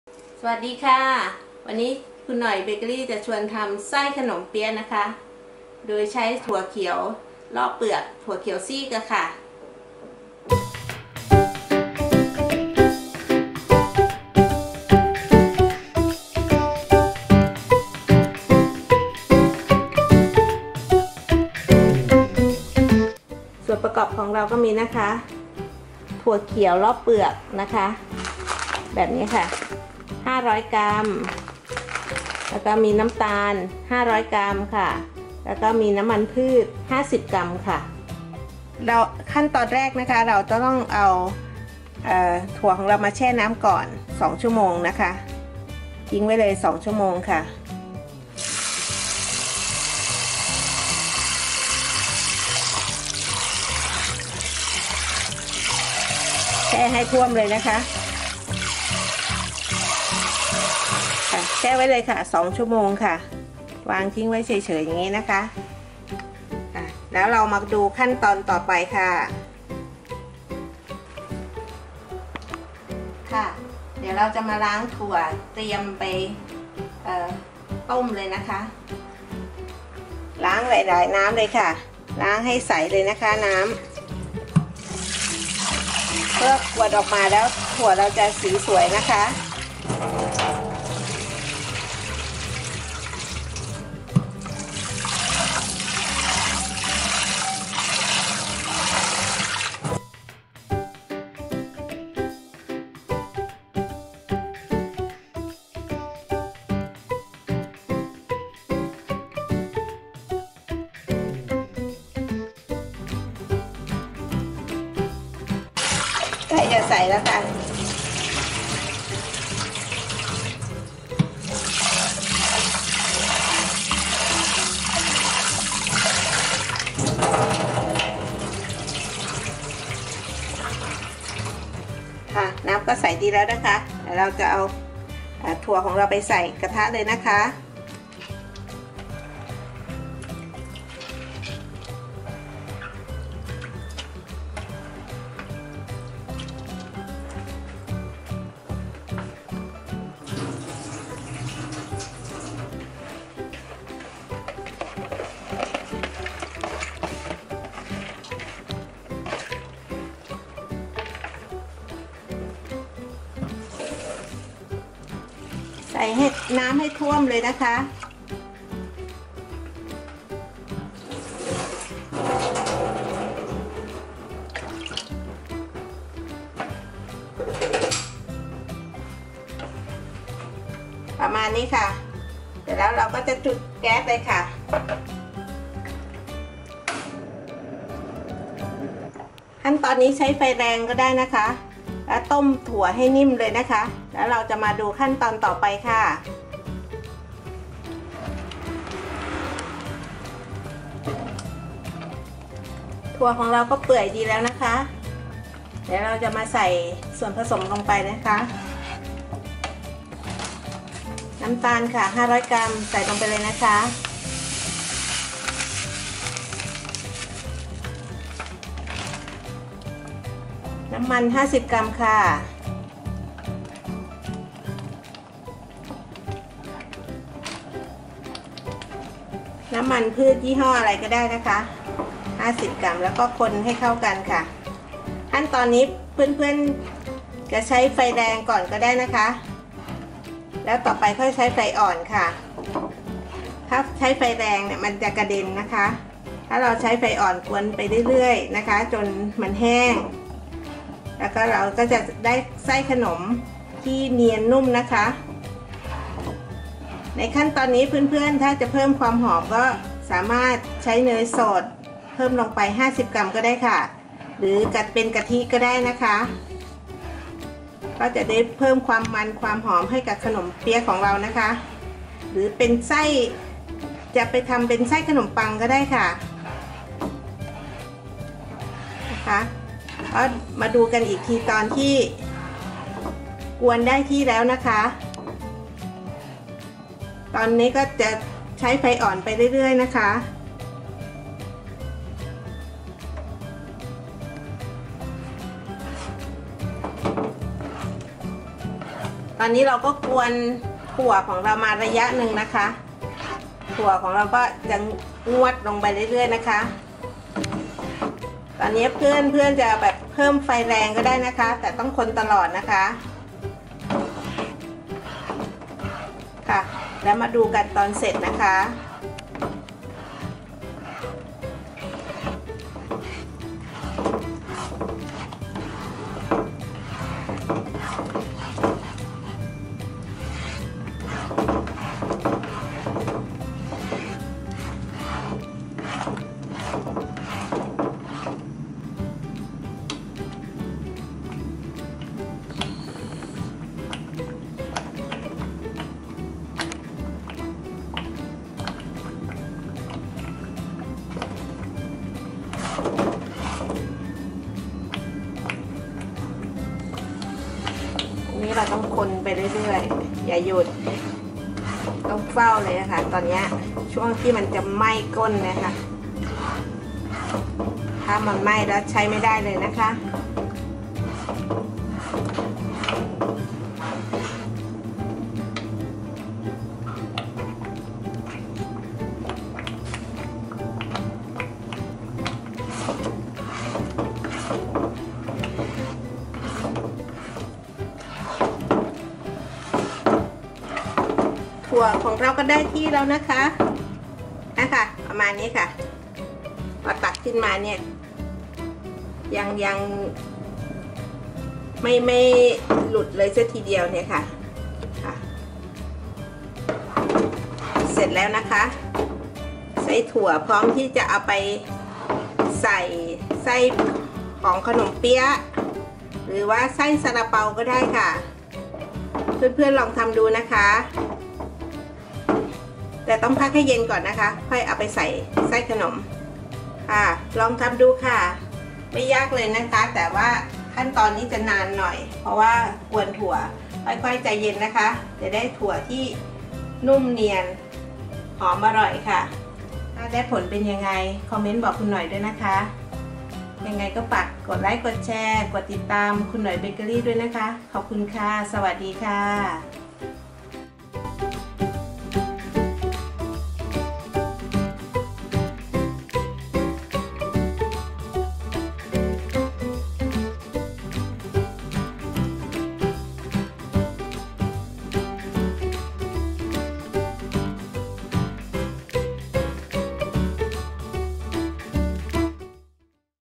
สอนทำไส้ขนมเปี๊ยะ - คุณหน่อยเบเกอรี่
สำหรับคลิปนี้คุณหน่อยเบเกอรี่จะมาสอนไส้ขนมเปี๊ยะ ใช้ไฟอ่อน และอาจจะใช้เวลานาน แต่จะได้ไส้ขนมที่นุ่มละมุน อร่อย ทำพายไส้ถั่วก็ได้ ทำขนมเปี๊ยะไส้ถั่วก็ดี ลองทำตามกันดูได้เลยครับ